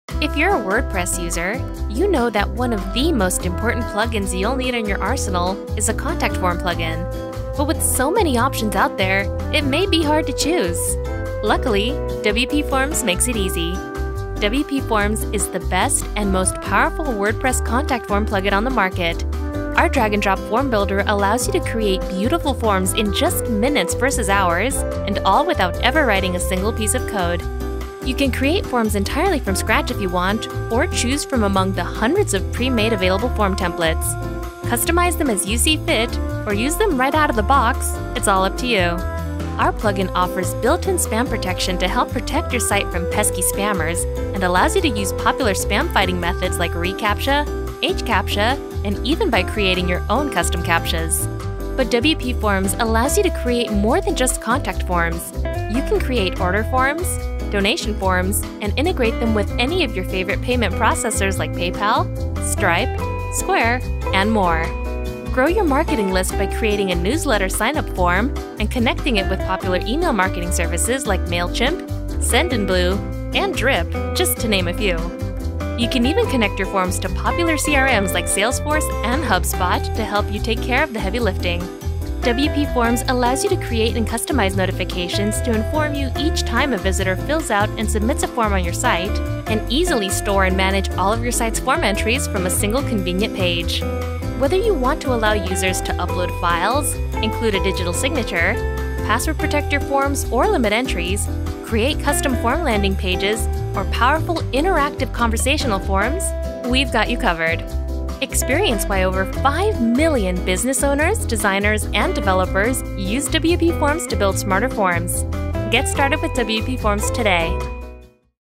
WPForms - the Best WordPress Contact Form Plugin
Welcome to WPForms, the best WordPress contact form plugin on the market.

In this video, we'll show you the tons of different features that make WPForms the most powerful and easy to use WordPress contact form plugin available.

Our drag and drop form builder allows you to easily create any kind of form with just a few clicks. And with our built-in spam protection, easy entry management, payment and email marketing addons, and conversion tools, you won't need to look anywhere else to help you create the most powerful forms on WordPress.

We took the pain out of creating online forms and made it easy.